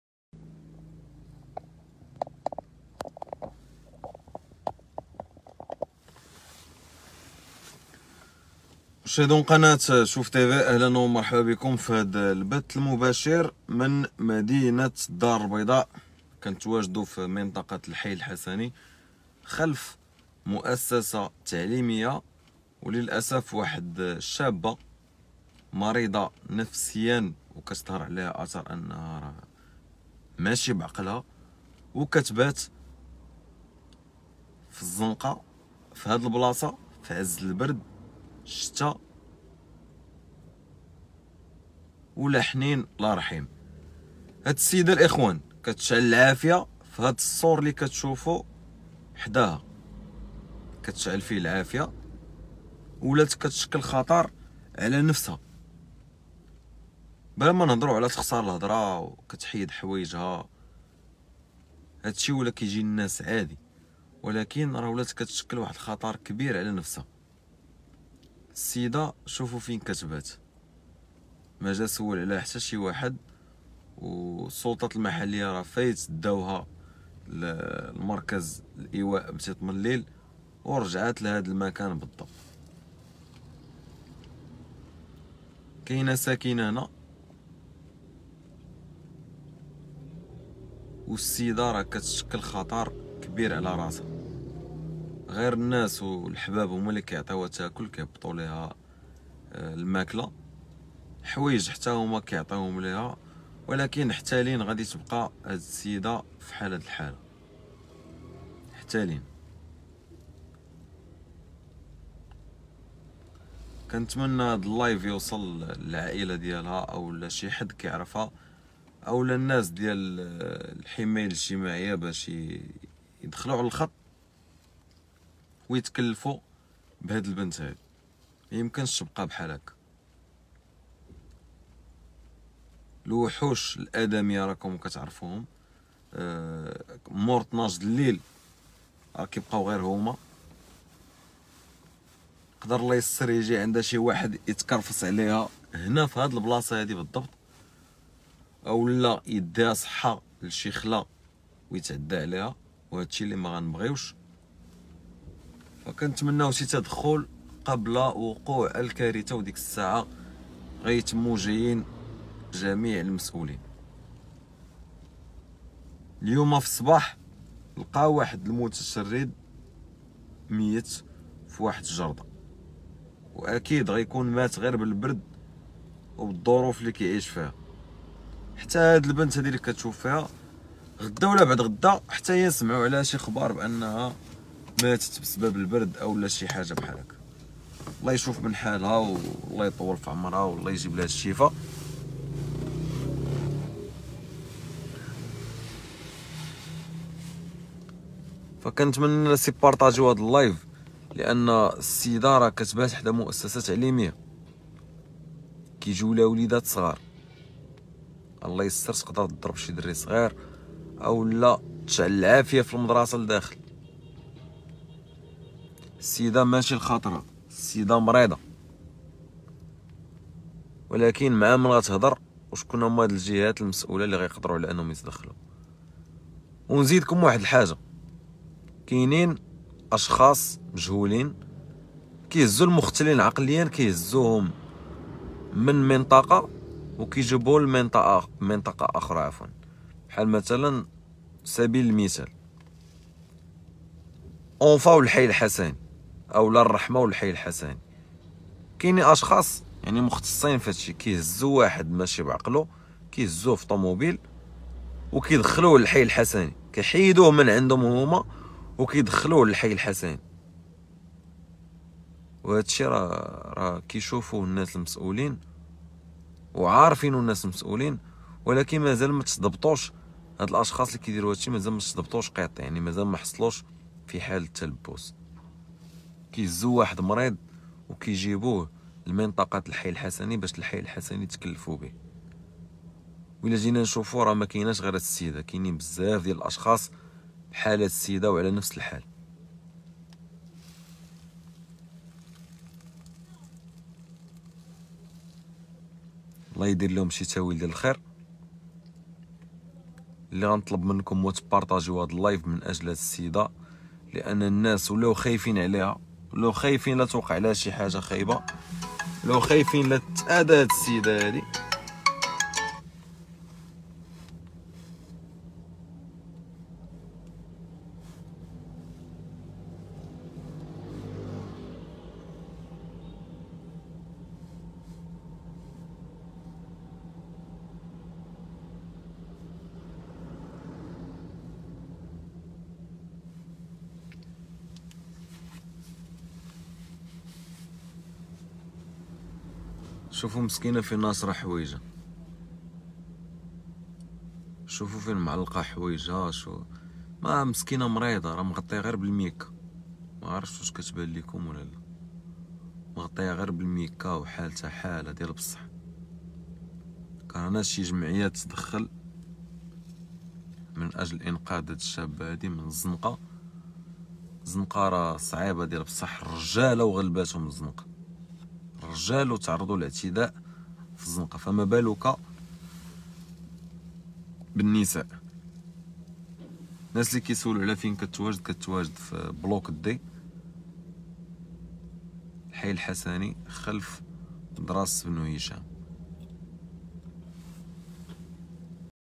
فعز الشتاء و البرد القارس..شابة مريضة نفسيا كاتسكن خلف مؤسسة تعليمية
مباشرة من الدارالبيضاء..فعز الشتاء و البرد القارس..شابة مريضة نفسيا كاتسكن خلف مؤسسة تعليمية و كاتشعل العافية و الناس كايخافو دير شي حاجة فراسها